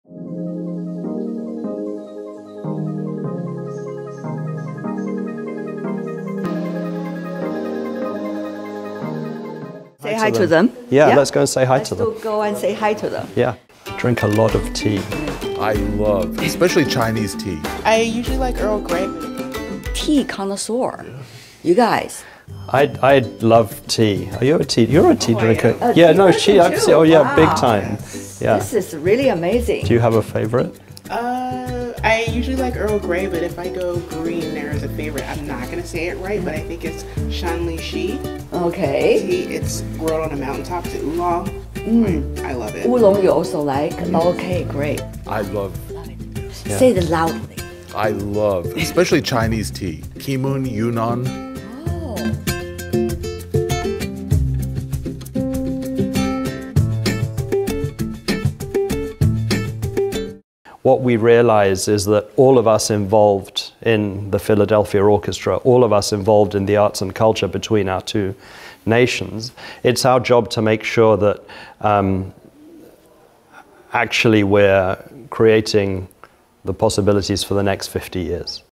President of the Philadelphia Orchestra: Brewing up more cultural ties in the next 50 years
The Philadelphia Orchestra recently paid a visit to China to celebrate the 50th anniversary of its first trip to China in 1973. In an interview with CGTN's Tian Wei, many of the Orchestra's musicians expressed their love for tea. Among these tea aficionados is Matías Tarnopolsky, President and CEO of the Philadelphia Orchestra. He said that the whole orchestra will create more possibilities for cultural exchanges between China and the U.S.